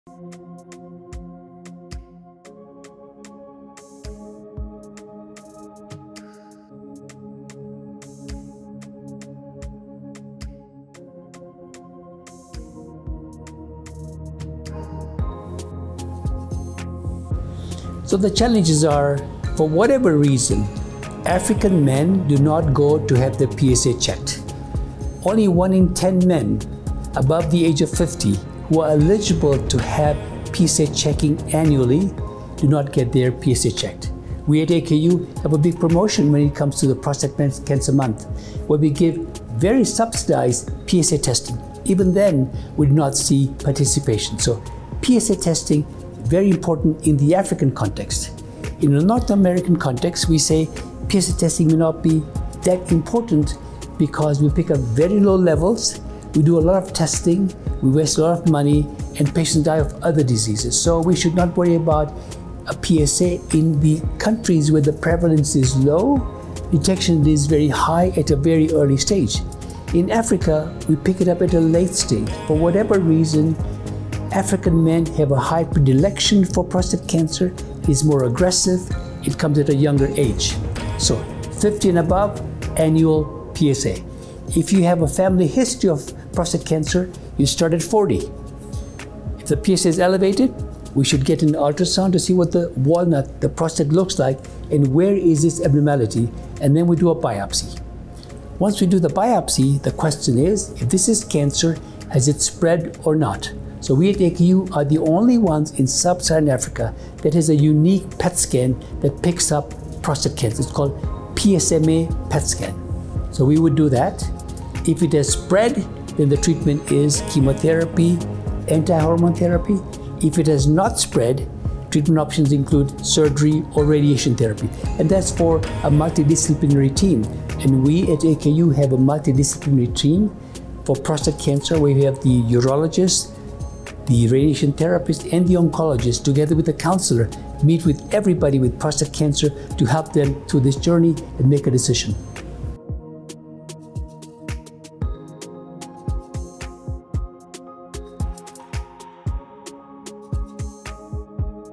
Critical Illness Insurance - What are the challenges of diagnosing Prostate Cancer?
Prof. Mansoor Saleh, Chair Dept. , Aga Khan University discusses Prostate Cancer.

Cancer is one of the Illnesses covered covered in the Critical Illness Cover that guarantees a cash lump sum payment on first diagnosis on any of the illnesses covered.